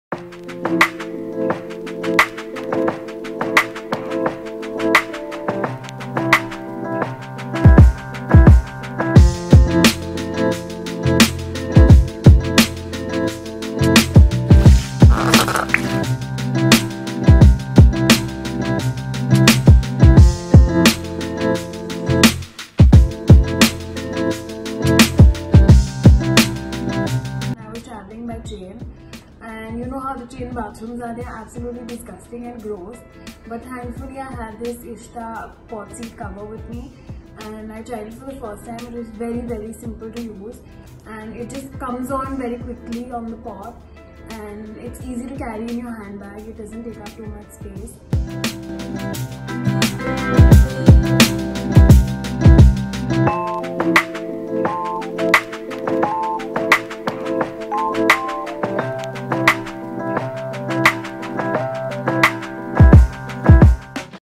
BrandsofIndia - Carry your Care with these Waterproof toilet seat covers from ISHTA Creation
There are about 50 bacterias per square inch on the toilet seat, which we could not see with our naked eyes, as per a study.

Using a public restroom can turn into a nightmare if you are not careful.

They are a hotbed for catching infections especially urinary tract infection which can affect the bladder and urethra, and in extreme cases kidney too.

There are ways that you can protect yourself from them like using toilet seat covers.

One such manufacturer is ISHTA Creation.

Made of non-tearable fabric, the disposable toilet seat covers have a waterproof coating and can fit any toilet seat.

They are easy to carry and come in 4'x4' small pack of 5 covers.

Its fold mechanism doesn’t allow any contact with the seat and doesn’t slip too.

If this is what you have been looking for head straight